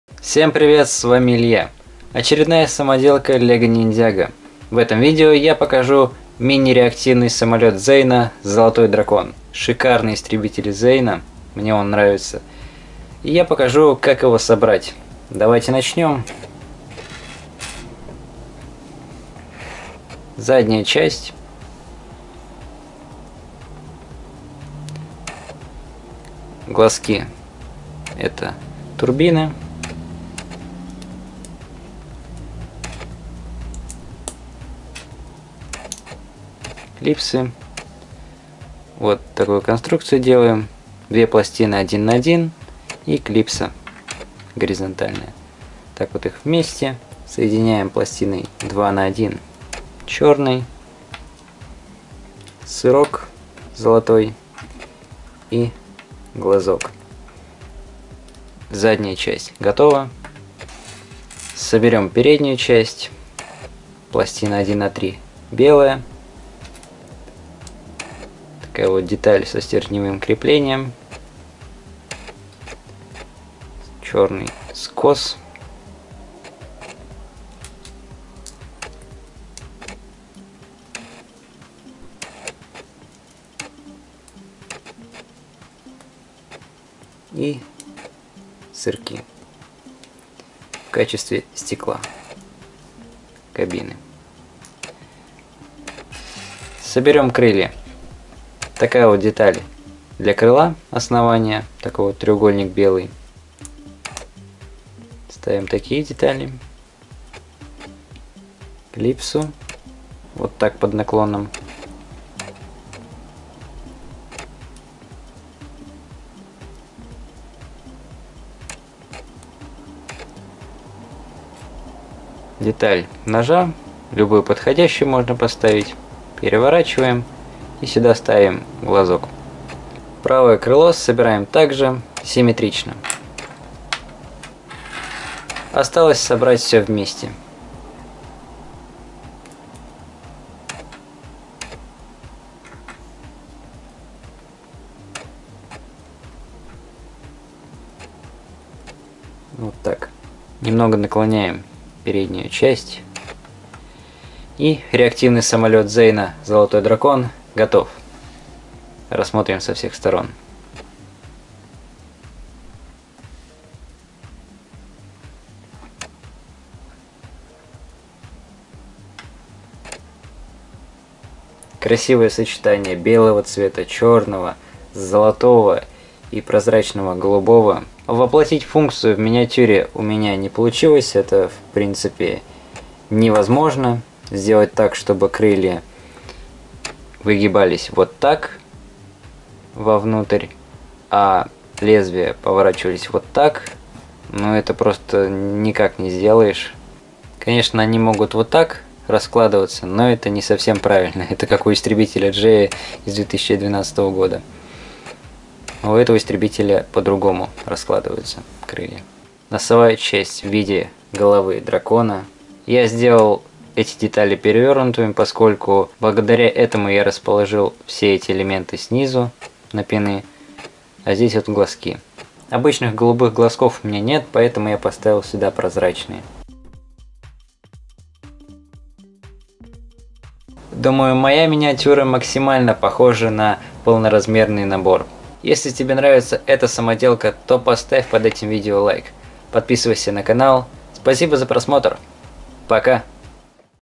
LEGO Ninjago MOC #35 Мини-Реактивный самолёт Зейна "Золотой дракон"
Истребитель Зейна из 16 сезона Ниндзяго в мини-формате!